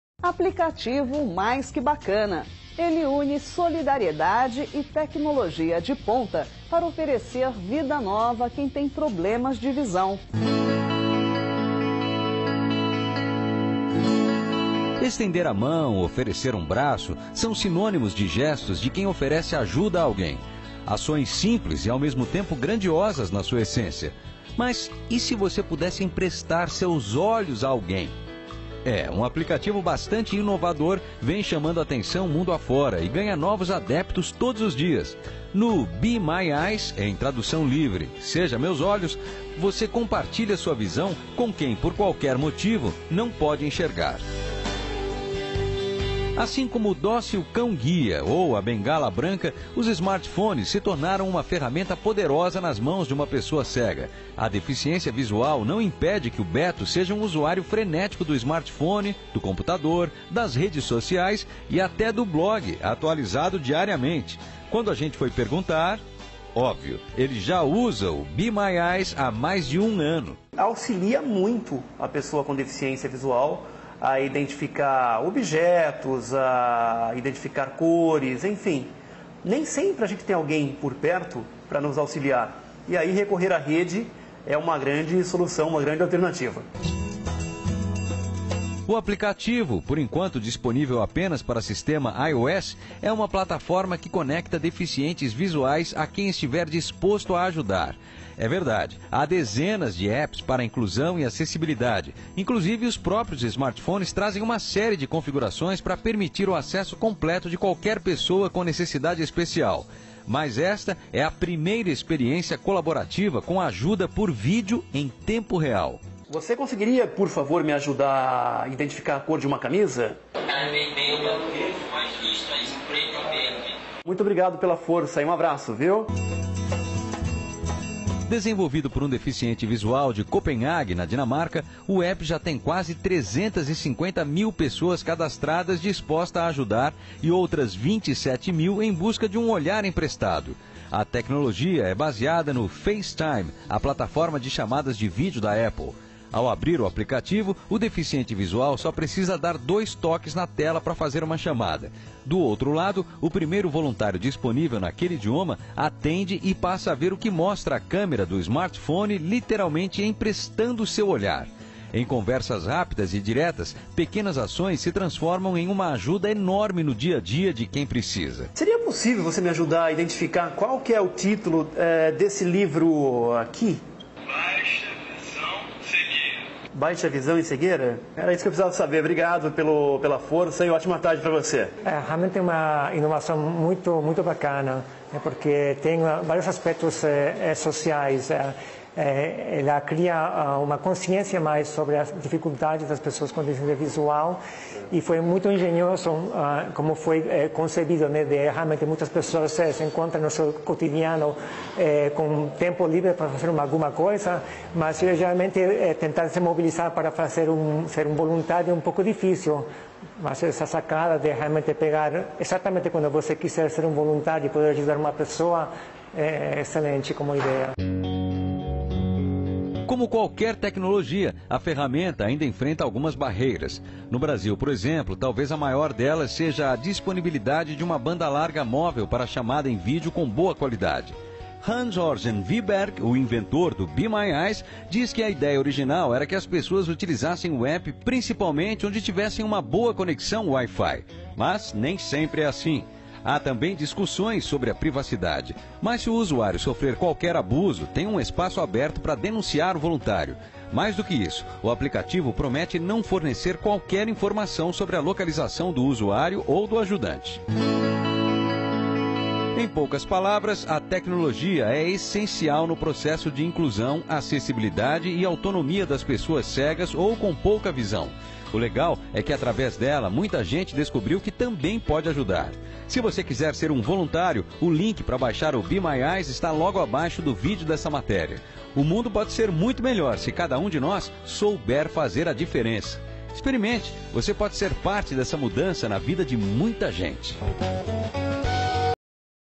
reportagem sobre o Aplicativo Be My Eyes seja meus olhos
Nós da Inclusão Ativa PCD compartilha essa reportagem sobre um recurso tecnológico que ajudam deficientes visuais no momento que se encontra sozinho. Note que legal você emprestando sua visão instantaneamente para um cego por meio do aplicativo “Be My Eyes” quer dizer “seja Meus Olhos!”, auxiliando essas pessoas ao redor do mundo!

Lembre-se, juntos podemos mudar o mundo!

• No canal do Gabriel Metzler explica a utilização desse aplicativo, acesse no link a seguir: